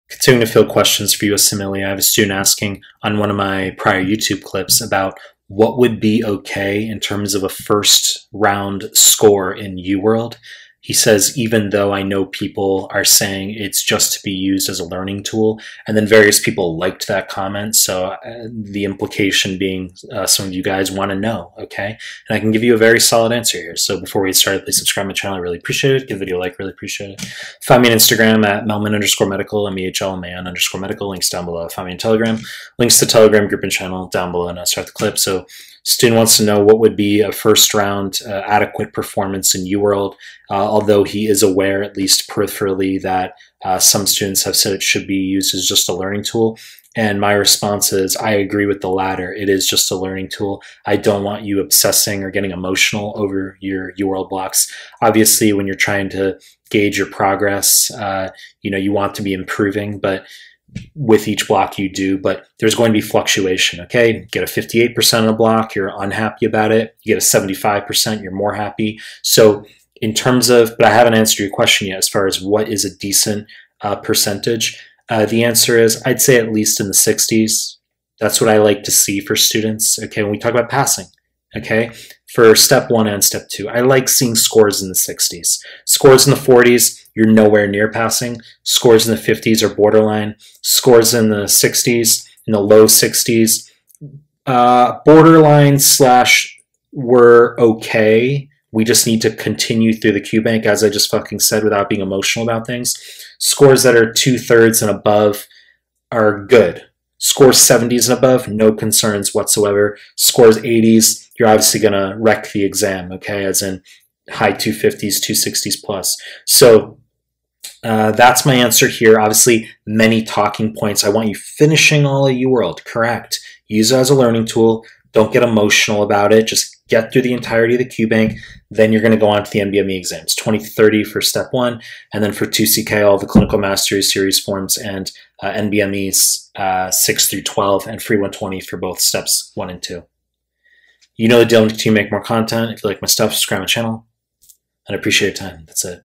"What is a good UWorld % correct for my blocks?"
In this clip I answer a student's question about what is an approximate "good" percentage to be scoring in Qbank / UWorld blocks.